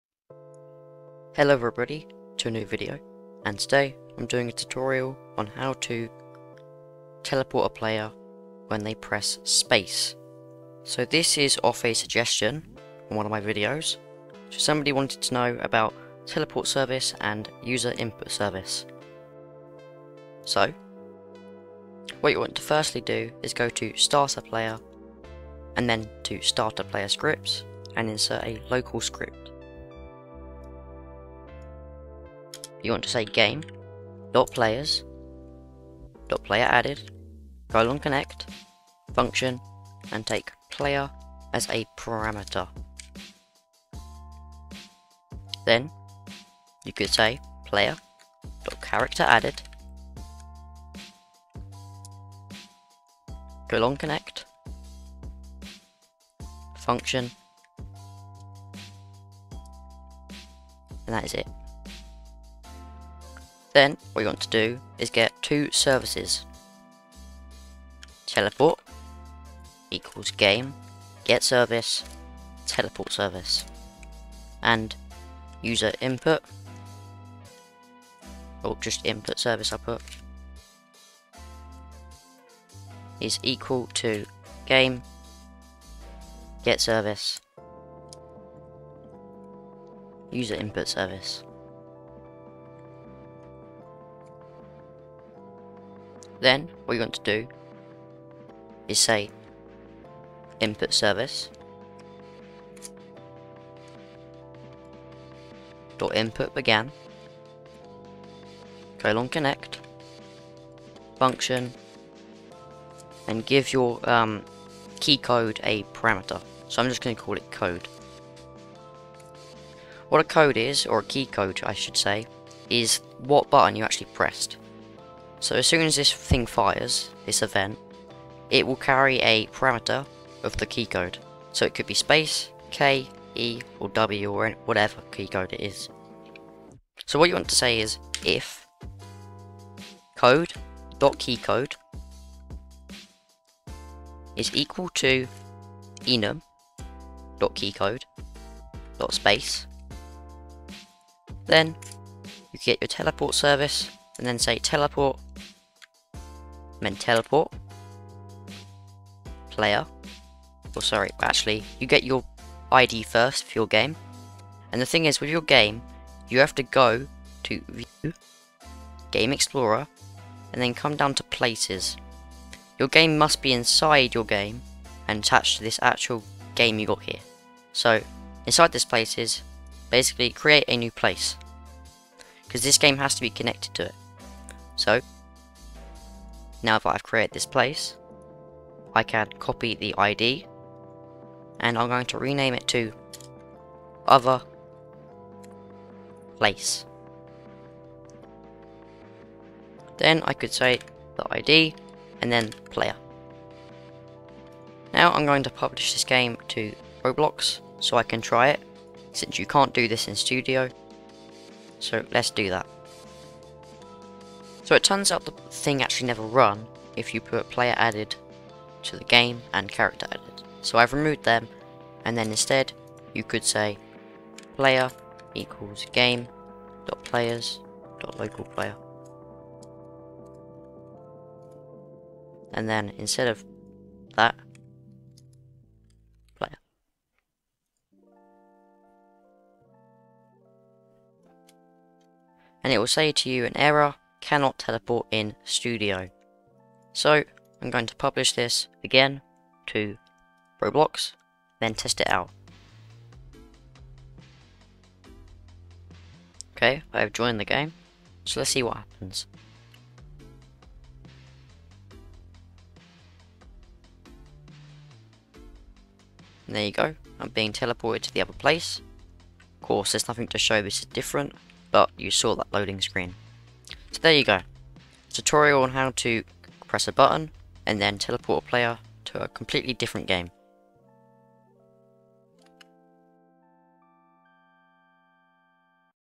Teleporting A Player (Roblox Studio Teleport / InputService Tutorial)
This video shows how to teleport a player using userinputservice and teleport service as a suggestion provided by giganticjohncena - yeah nice name dude lol.